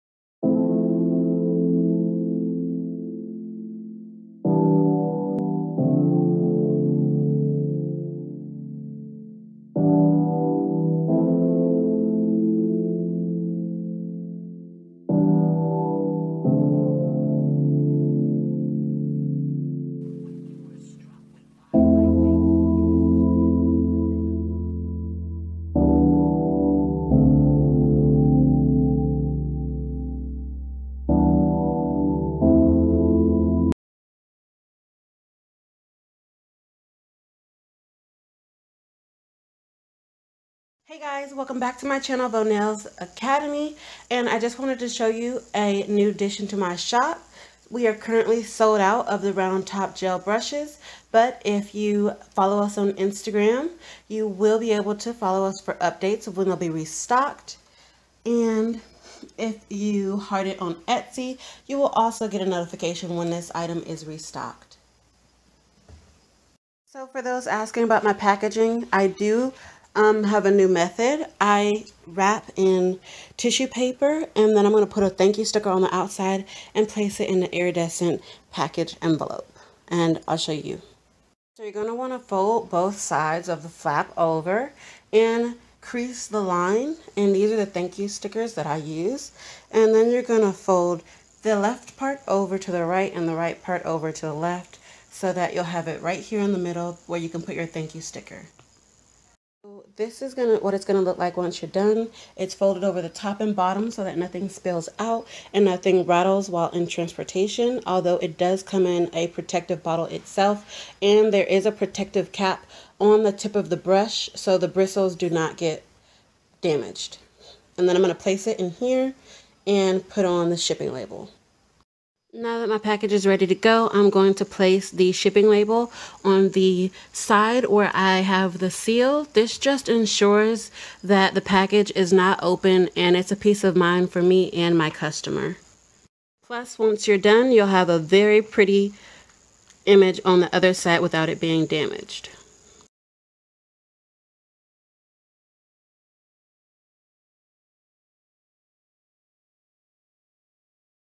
How I Package My Orders (Part 1) | Round Top Gel Brushes
This video is to show an addition to my shop. Round Top Gel Brush. Also, I wanted to show the new method I have for packaging my orders.

Get Your Custom Press On Nail set today! Or see cute nail accessories and supplies. More coming soon!

Round Top Gel Brush
SOLD OUT

*NEW SET*

P.s I did not forget about the 420 video I am working on it right now.